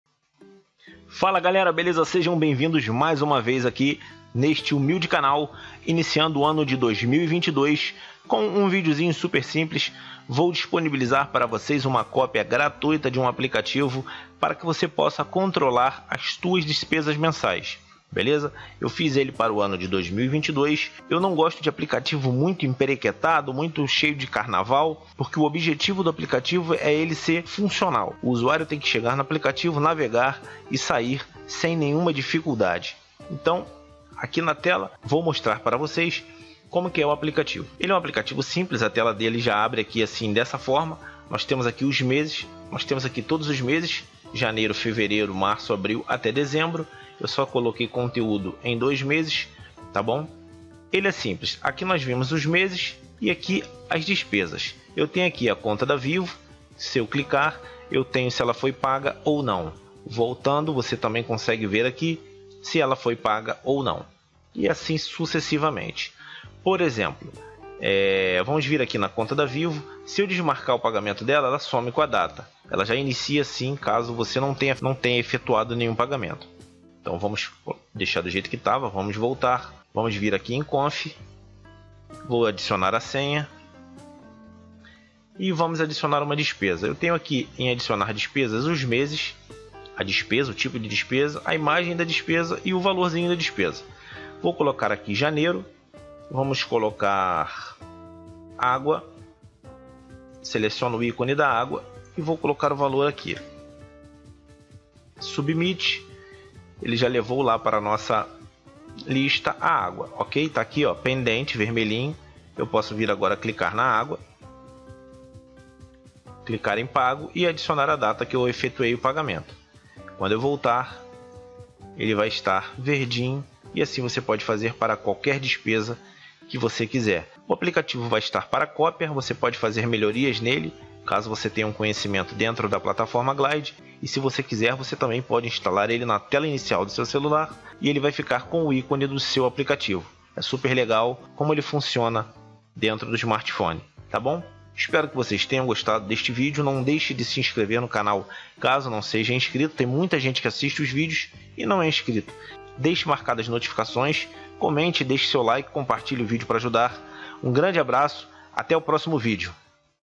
GLIDE APP 🎁 PRESENTE DE ANO NOVO
Está plataforma Glide App está cada vez melhor. Crie aplicativos no-code de forma simples e fácil. E se quiser se aprofundar, mande uma mensagem no meu whatsapp e pegue um desconto para o curso completo Glide 2.2

Deixe seu comentário sobre o apk, aí!